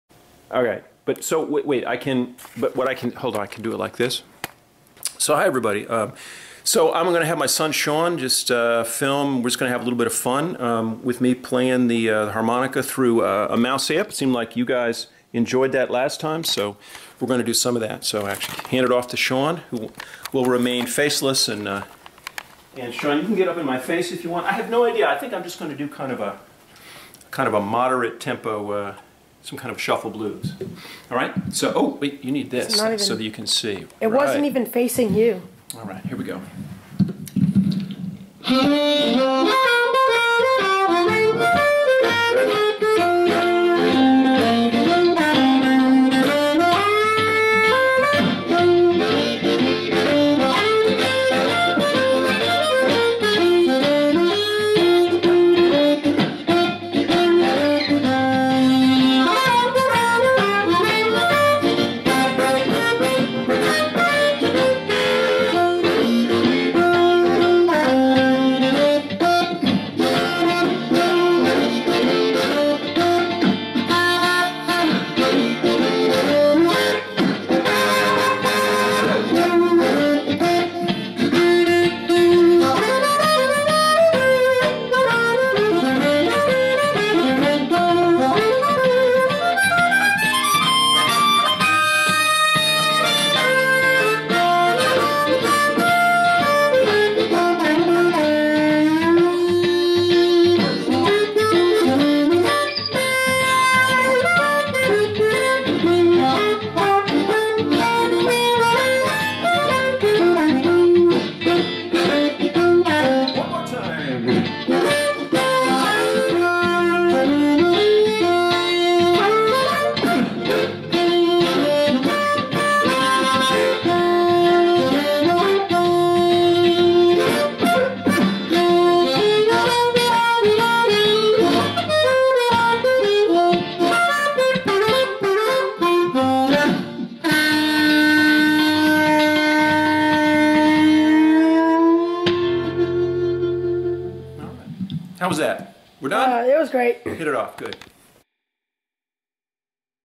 Labor Day livingroom harmonica blues jam - Gussow & son
B-flat harp.  The guitar was added in post-production (Garage Band).

Not much more to say about this one!